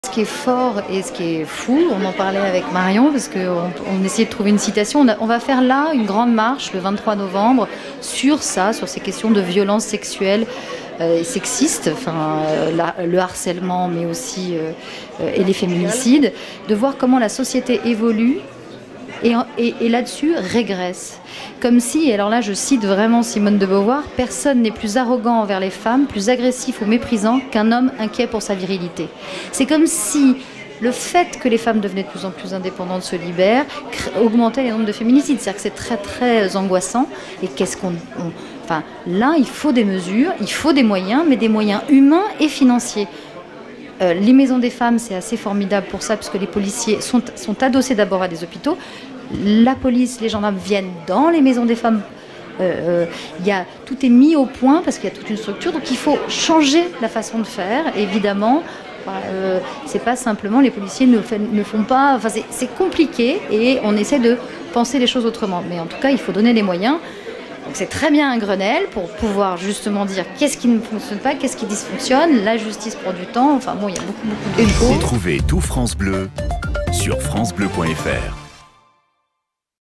Julie Gayet réclame plus de moyens pour lutter contre les féminicides
Invitée en direct sur le plateau de France Bleu Périgord à l'occasion du 28e festival du film de Sarlat, Julie Gayet a évoqué son combat contre les violences conjugales. L'actrice et réalisatrice réclame plus de moyens pour lutter contre les féminicides en France.